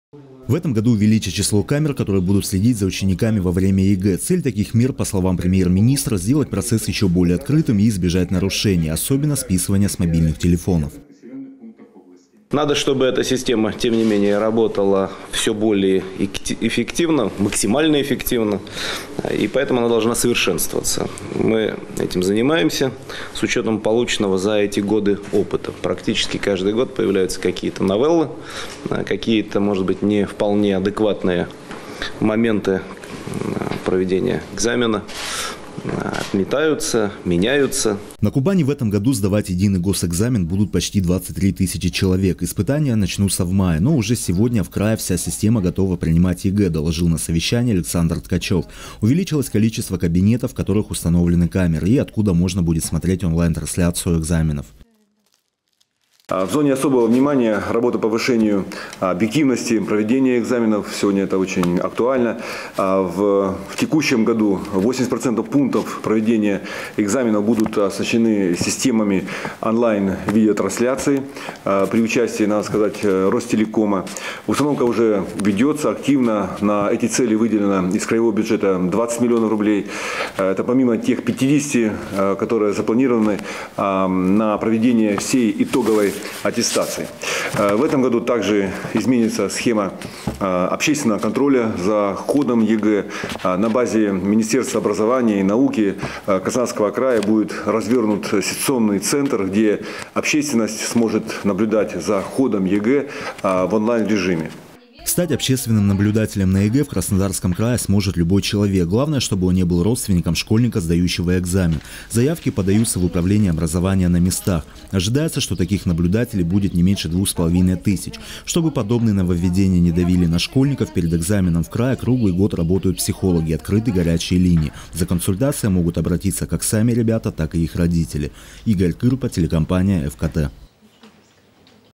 Следить за сдачей ЕГЭ будут пристальней. Новости 24 Сочи Эфкате
Об этом сегодня шла речь во время селекторного совещания, которое с регионами провёл Дмитрий Медведев. В обсуждении готовности Краснодарского края к сдаче вступительных испытаний принял губернатор Александр Ткачёв.
Сочи. Новости.
Телеканал Эфкате.